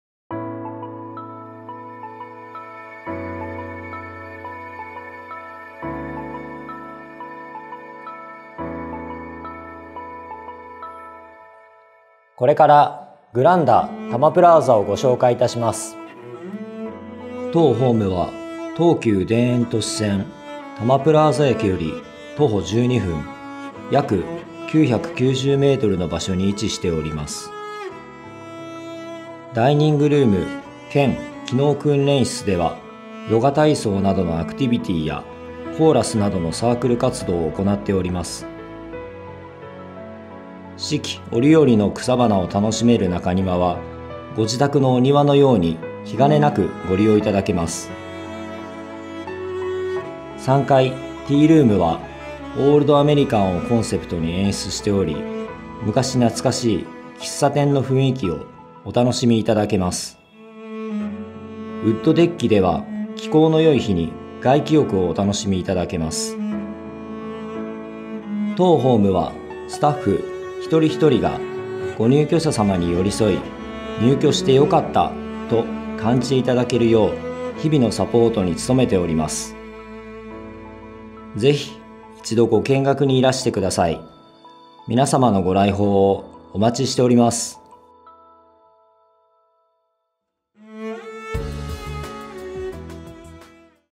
「グランダたまプラーザ」の施設案内 - 住宅型有料老人ホーム【いい介護】
「グランダたまプラーザ」は、お買い物に便利な「たまプラーザ駅」が最寄り駅。周辺の街並みは庭園のような美しさです。ミニキッチン付きのお部屋でご自宅と同じような暮らしをお送りいただけます。

■入居時費用：140.0万円〜
■月額費用　：20.9万円〜
■入居条件　：自立・要支援・要介護・認知症可
■施設住所　：神奈川県川崎市宮前区犬蔵2-34-15
■アクセス　：東急田園都市線「たまプラーザ駅」より徒歩12分（約990m）
■運営会社　：株式会社ベネッセスタイルケア

チャプターリスト
0:00 イントロダクション
0:11 お客様相談担当
0:17 アクセス・立地
0:29 ダイニングルーム兼機能訓練室
0:40 中庭
0:50 ティールーム
1:01 ウッドデッキ
1:08 施設の心構え
1:21 見学のご案内

今日明日の見学予約も受付中！　土日祝日も見学できます！